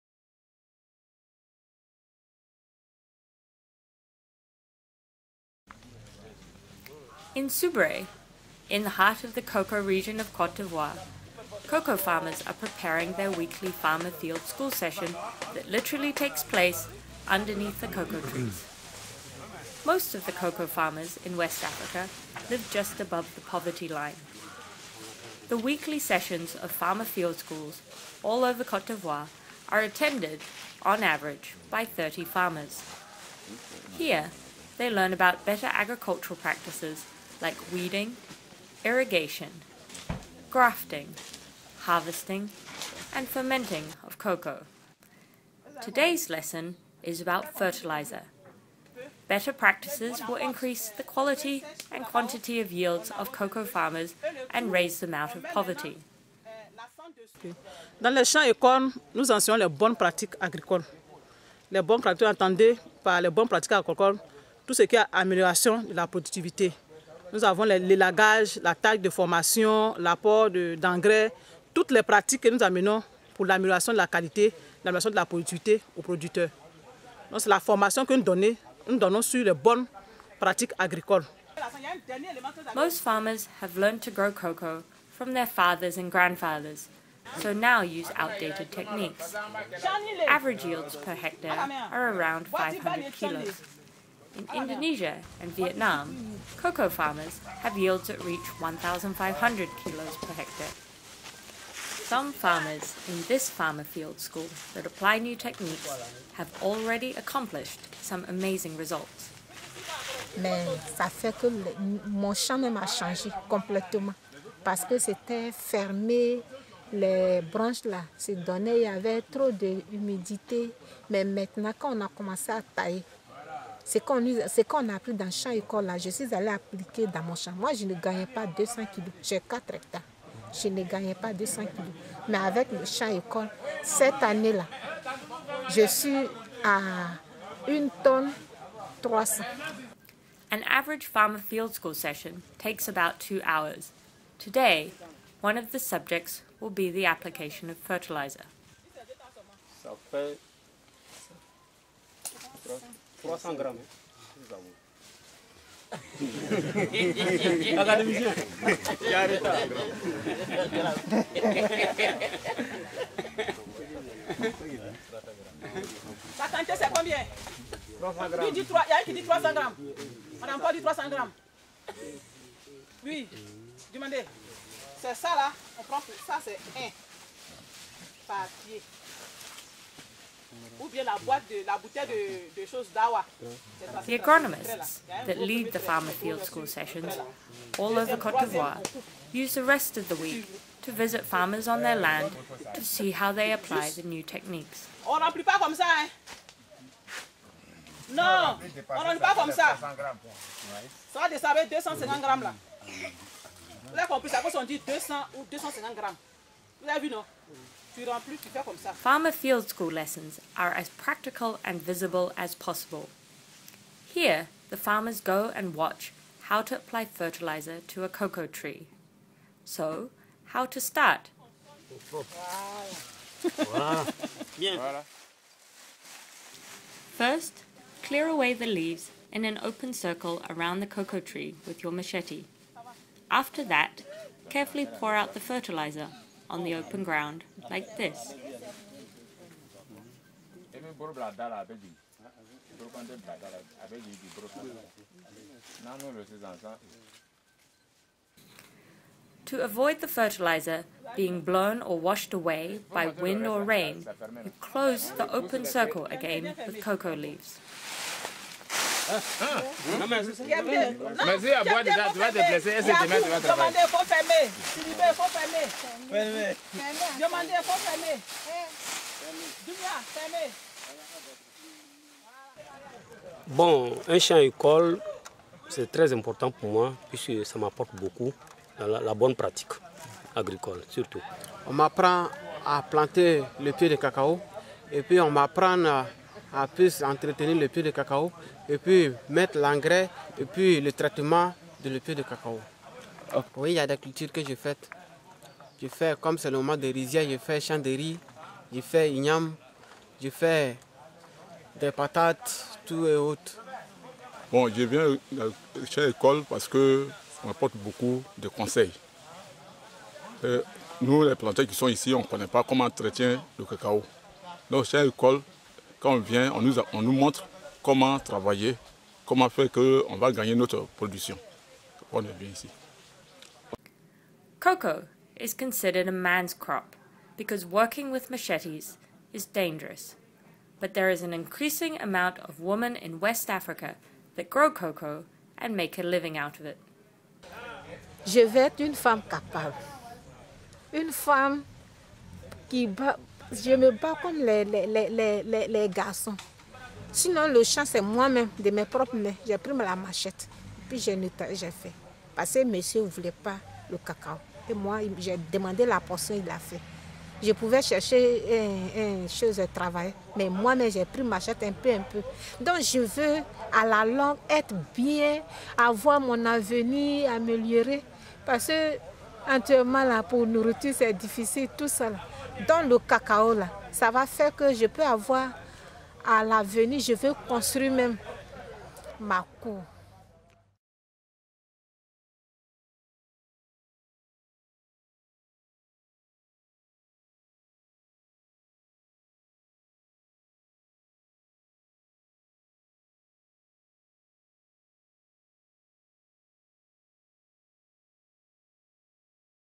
Cocoa Farmer Field School in Cote d'Ivoire
A cocoa farmer field school in Cote d'Ivoire demonstrates good agricultural practices learned and the correct application of fertilizer to improve their yields and increase sustainable production.